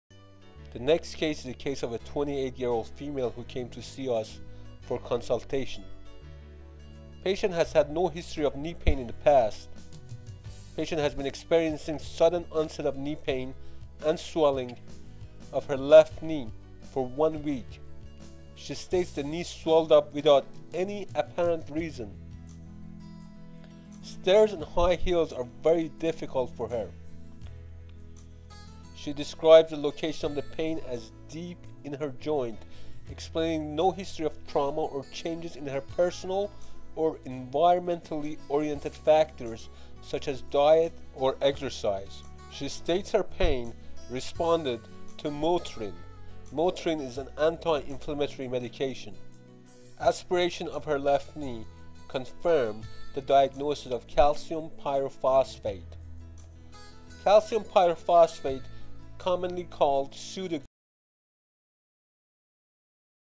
60. Farshchian: Dr Alimorad Farshchian discusses arthritis.
A renowned physician at The Center for regenerative Medicine, Dr Farshchian has published a second book on Orthopedic regenerative series. Dr Farshchian's clinical and research interests have been in the areas of the development of non surgical procedures for prevention and regeneration of sports injuries.